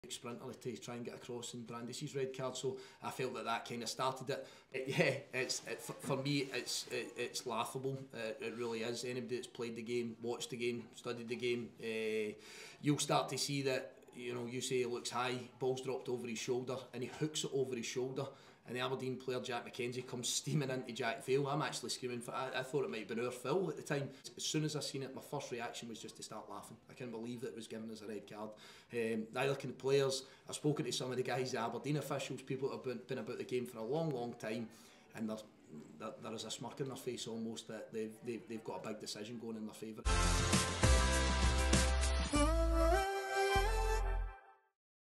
Stuart Kettlewell reacts to Jack Vale’s red card: ‘For me it’s laughable’
🗣️ ‘For me it’s laughable’

Stuart Kettlewell reveals that he believes Craig Napier made a mistake sending off Jack Vale, and that it could have been a Motherwell free kick.

Buying a car?